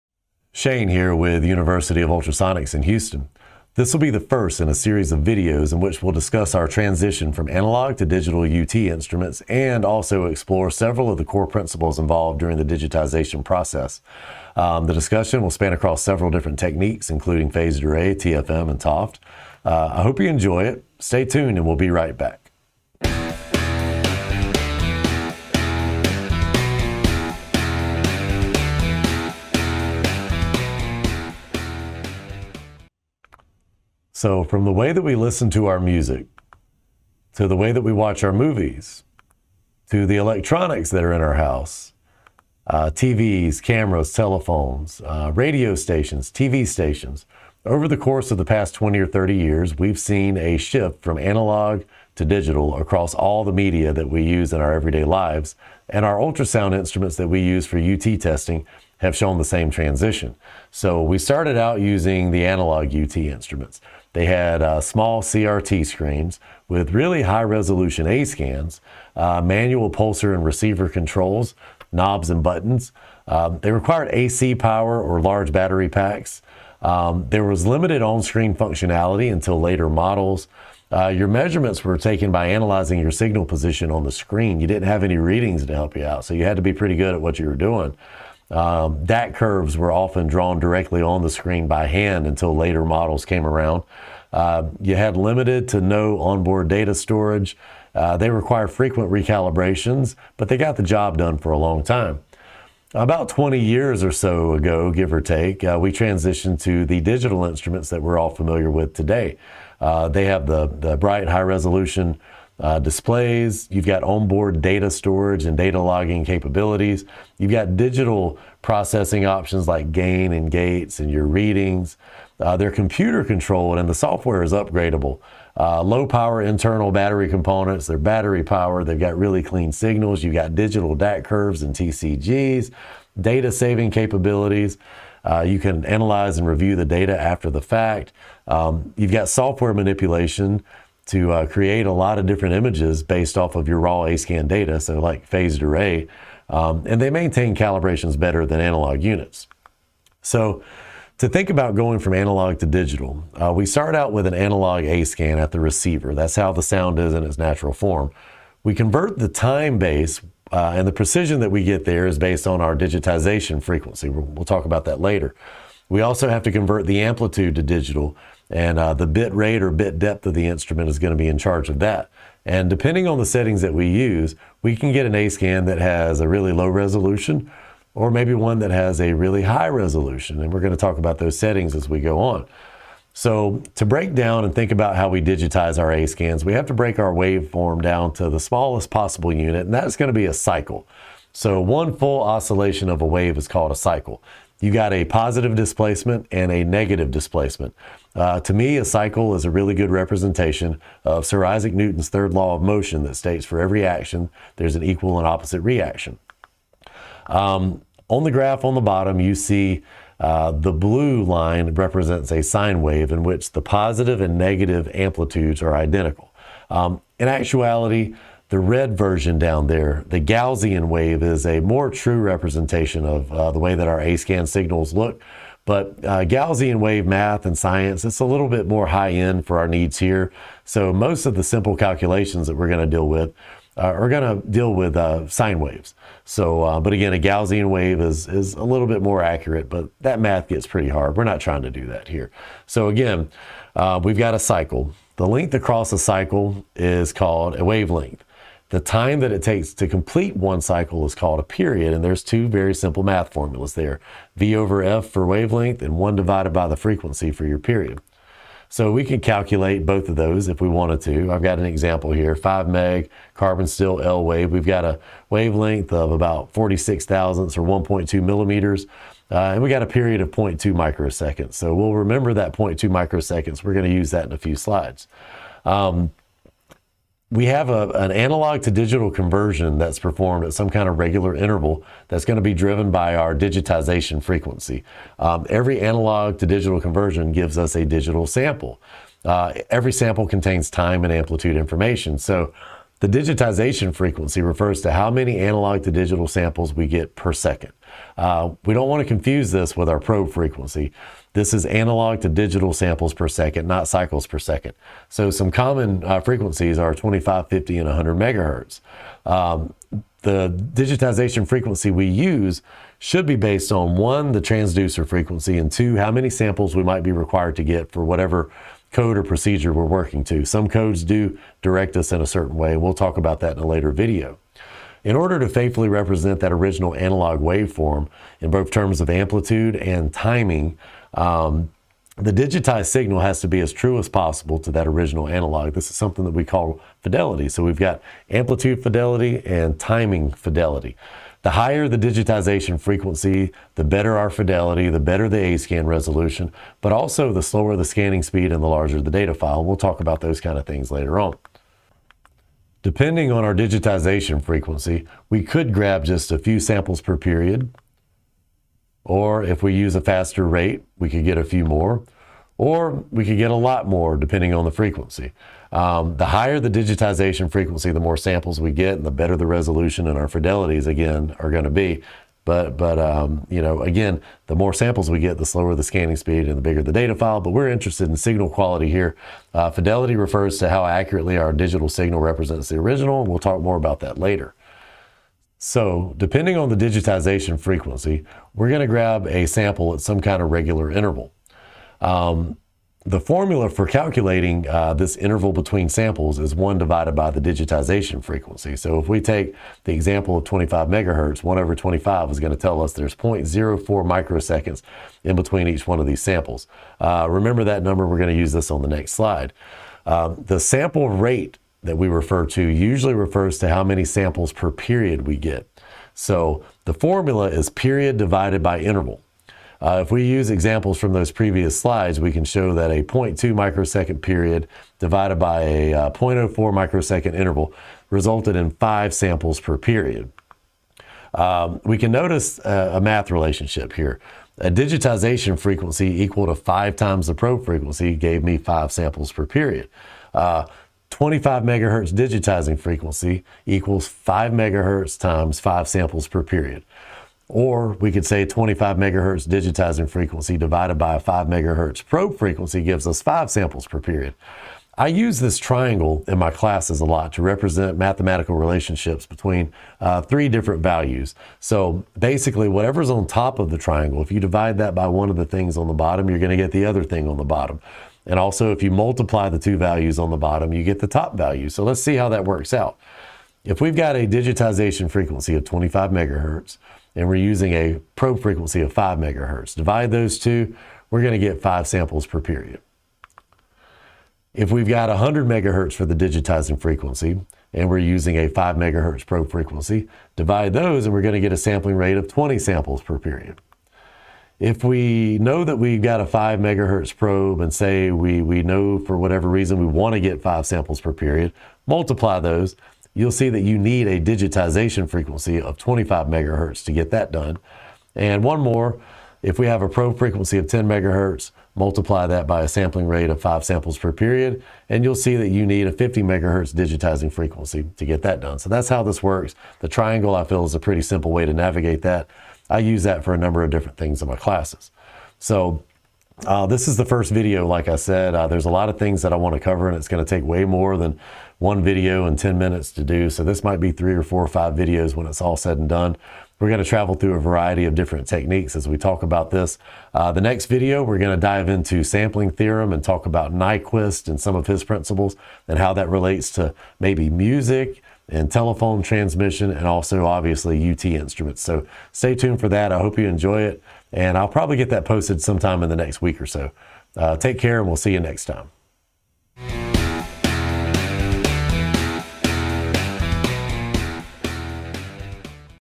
The Digital Revolution- Part 1- Intro to Digitization
In this first video of the "Digital Revolution" series, Shane Walton with University of Ultrasonics introduces us to a few of the digitization principles associated with UT testing instruments. Digitization Frequency, Sampling Rate, and other topics are discussed.